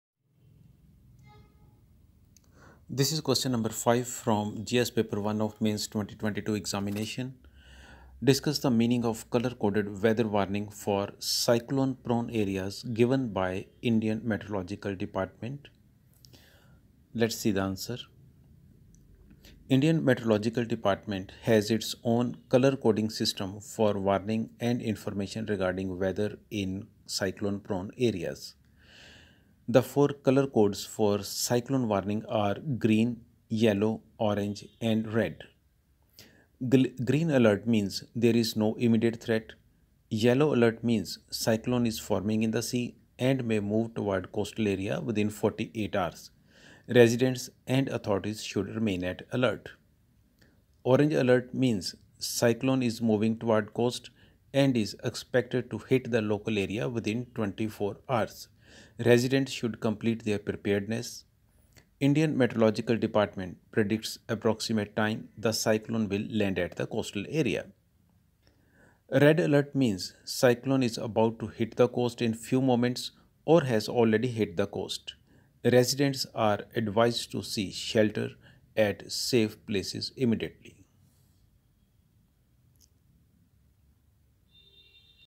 Discuss the meaning of colour coded weather warning for cyclone prone areas given by Indian Meteorol
CSE upsc ias  mains GS1 2022

Q.5 .Discuss the meaning of colour coded weather warning for cyclone prone areas given by Indian Meteorological Department